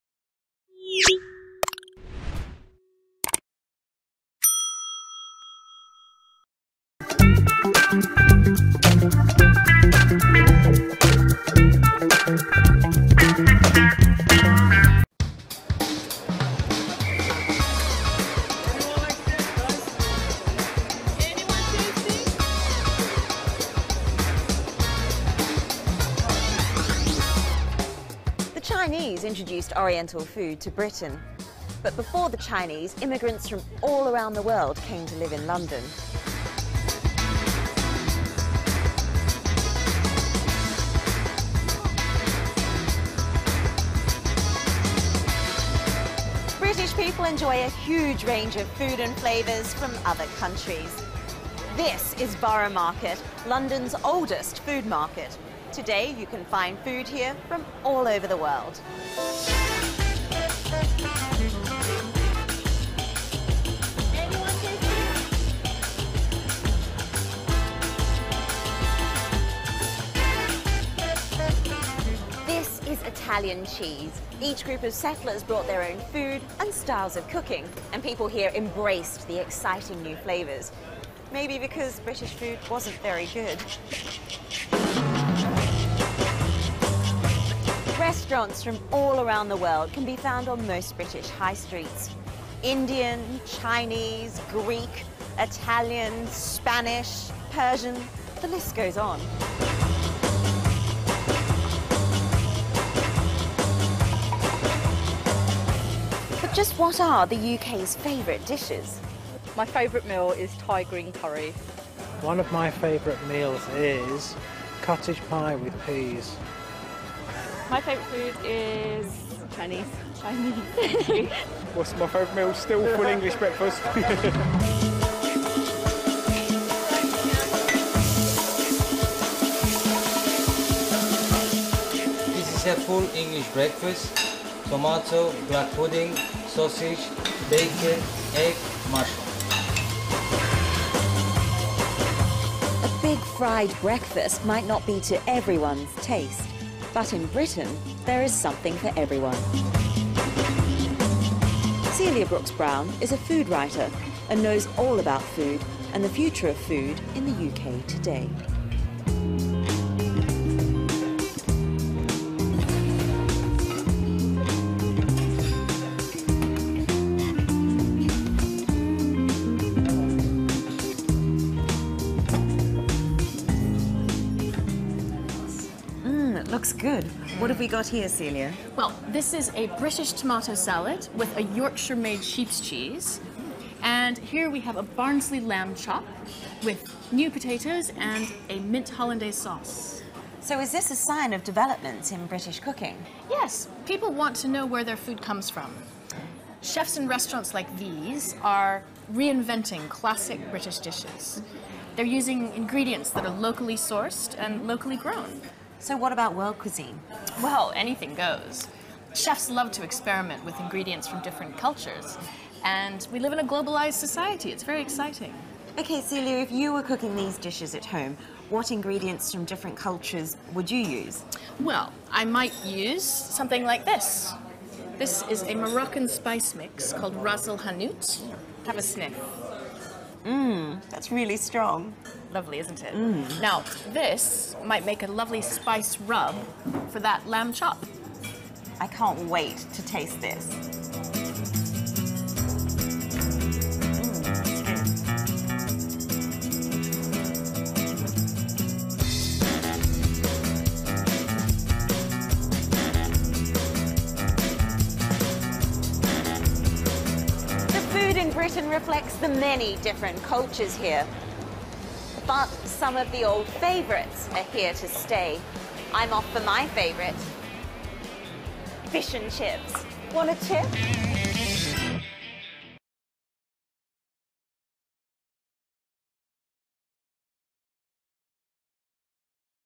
Підготовка до ЗНО з англійської мови аудіювання (Listening) #10
Підготовка до ЗНО з англійської мови аудіювання (Listening). Підготовка до ЗНО

Для того, щоб полегшити підготовку до зовнішнього незалежного оцінювання, а також ознайомити майбутніх учасників із складністю завдань, публікуємо завдання, котрі схожі з офіційними.

Прості правила для виконання завдання:
1. Зробити підготовче (Preparation) завдання;
2. Переглянути відео (можна разом з субтитрами);
3. Виконати інші завдання;
4. Переглянути правильні відповіді для самоперевірки.

Відповіді до тесту:

1. Preparation: picture matching
cheese curry peas full English breakfast
tomatoes lamb chops potatoes chips

2. Check your vocabulary: gap fill – countries and nationalities
1. (Great) Britain / the UK
2. Morocco
3. India
4. China
5. Greece
6. Italy
7. Spain
8. Thailand

3. Check your vocabulary: matching - food
1. c
2. a
3. h
4. b
5. e
6. d
7. f
8. g